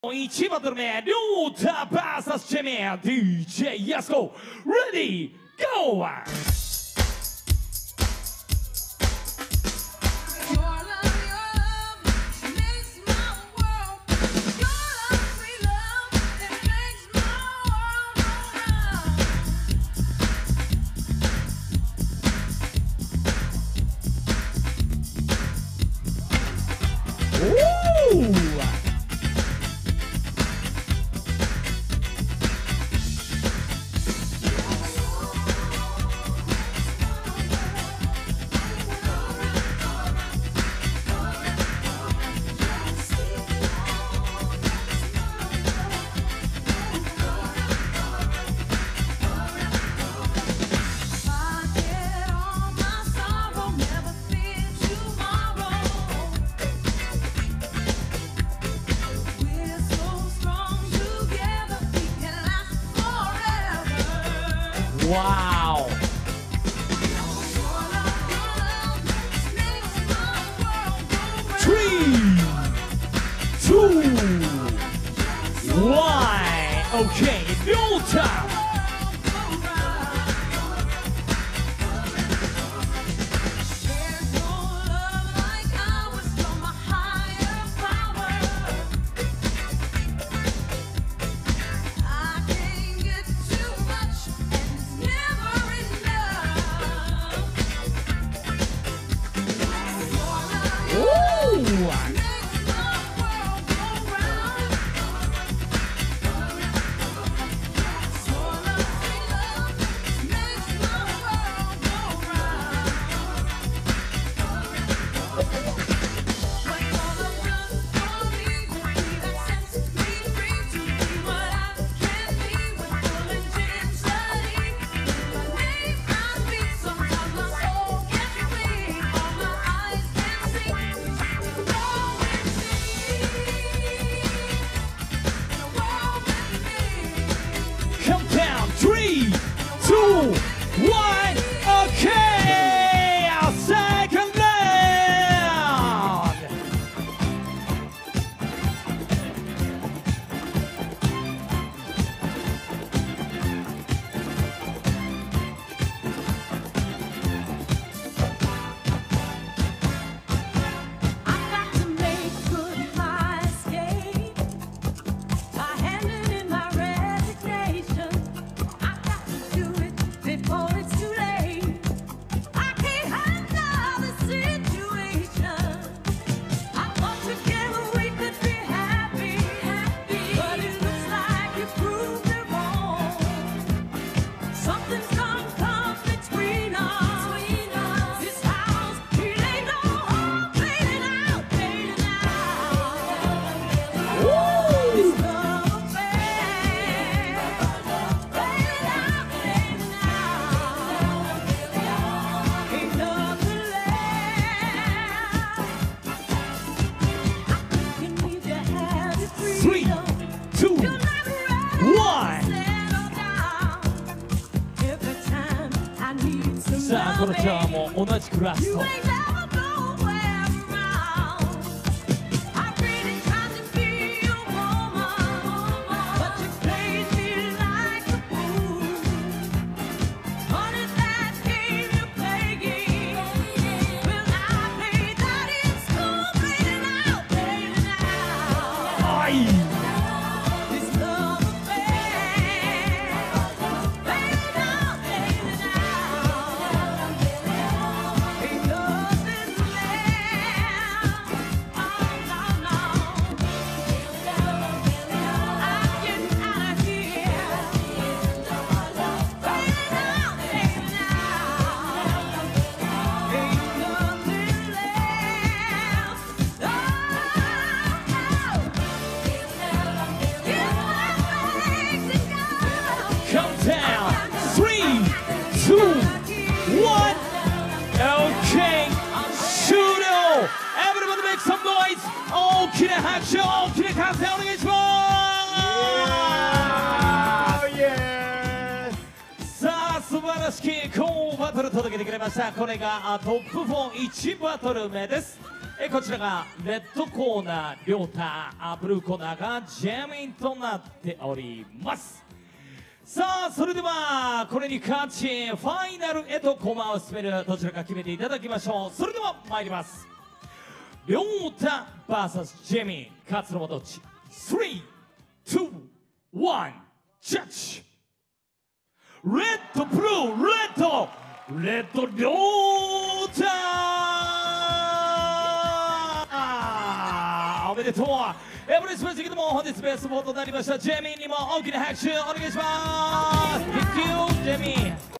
"RYO-TA vs JEMIN" OLD SCHOOL NIGHT VOL.25_2024_WAACKING 1vs1 BATTLE SEMIFINAL
オールドスクールバトルの祭典 "OLD SCHOOL NIGHT VOL.25"
2024.5.5 at ZEPP NAMBA
OSN VOL.25 WAACKING 1vs1 BATTLEに242人が参戦！

▼WINNER
Jeem（KOREA）

▼2nd PLACE
RYO-TA（兵庫）

▼TOP4
JEMIN（KOREA）
JUNKOO（東京・高知）

▼TOP8
ryj.（岡山）
Dada（Thailand）
You-ki（神奈川）
Ta-Bo-（大阪）

▼TOP16
HIKARINGO（大阪）
MIKUMIKU（長崎）
LEEVER（KOREA）
Ayanocchi（大阪）
MEME（大阪）
櫻子（静岡・東京）
Suzanne（FRANCE）
莉多（兵庫）